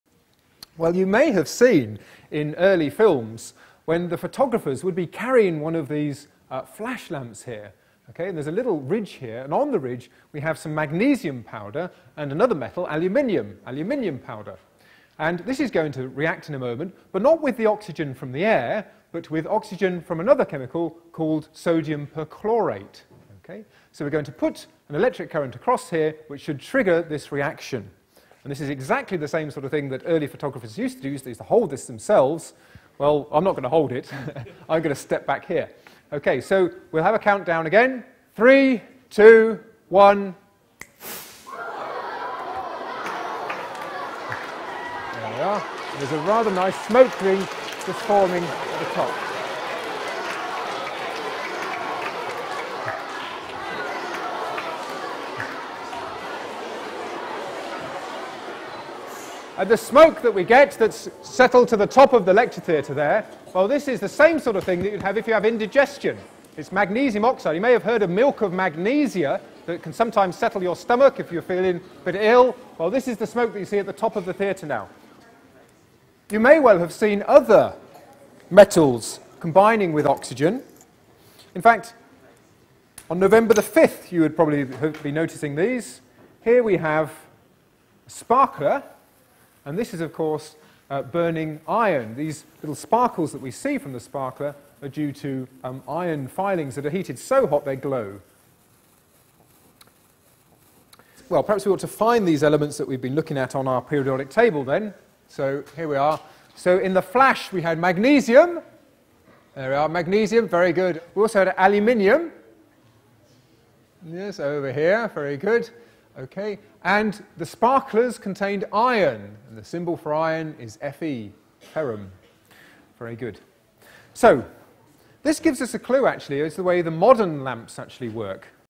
The Chemistry of Light 14 - Magnesium Flash Powder
An old-fashioned flash-lamp uses magnesium to light up a photograph.

From the Peter Wothers lecture - The Chemistry of Light